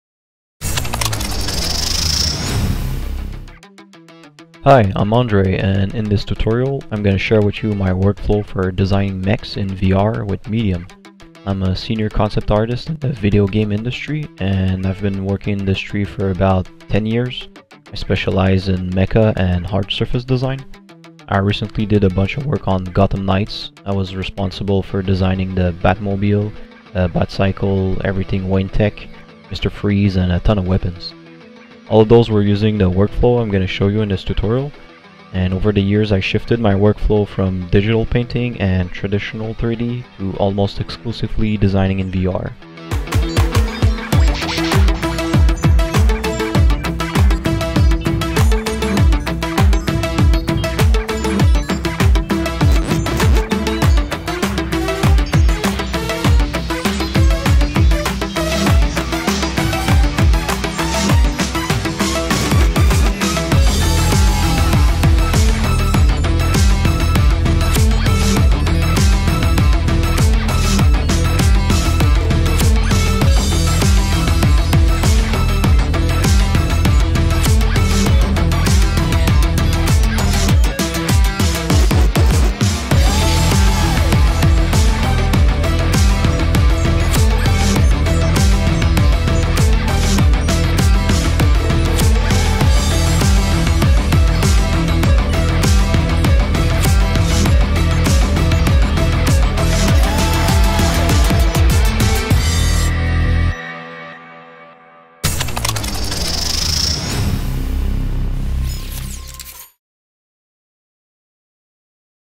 Designing Mechs in VR Using Medium With Andre-Lang Huynh
Learn how to use Medium to quickly and efficiently craft hard-surface mecha designs for the videogame industry with Andre-Lang Huynh

A WORKFLOW BUILT FOR SPEED & FLEXIBILITY

Learn how to use Medium to quickly and efficiently craft hard-surface mecha designs for the videogame industry. Senior Concept Artist Andre-Lang Huynh has worked in video games for a decade, specializing in mecha and hard-surface design. The workflow taught in this workshop is the same one used by Andre to design characters, vehicles, and weapons for Gotham Knights, including the Batmobile and Batcycle.

In this 3.5-hour workshop, Andre-Lang shares his VR workflow in great depth and demonstrates the flexibility of sculpting in VR as well as the precision that you can achieve with simple yet effective tools. You’ll see exactly how he uses Medium to block out and refine his mecha before rendering the final design using KeyShot.

This tutorial is designed to be enjoyed by everybody from aspiring artists to industry veterans looking to modernize their approach and add the power of VR to their arsenal. After completing this tutorial, artists should be able to quickly use Medium and KeyShot to create tangible and functional designs of their own. As a concept artist whose workflow has evolved from entirely 2D through adding 3D and is now almost exclusively VR, this is a fascinating workshop for 2D and 3D artists alike who are looking to add to their skillset and master VR sculpting tools.

The workshop also provides helpful brushes and kitbash parts to support the sculpting of mechanical and hard-surface designs with ease. Project files include Andre’s brush pack, kitbash pack, Medium scene file, and KeyShot materials.

CHAPTERS:
— Introduction to the VR Sculpting Workflow
— Gathering References
— Basic Tools in Medium
— Blocking out the Frame
— Building the Armored Shell
— Detailing Pass
— Polish & Focus Pass
— Kitbashing Workflow in Medium
— Posing & Exporting the Mech
— Rendering in KeyShot
— VR Sculpting Outro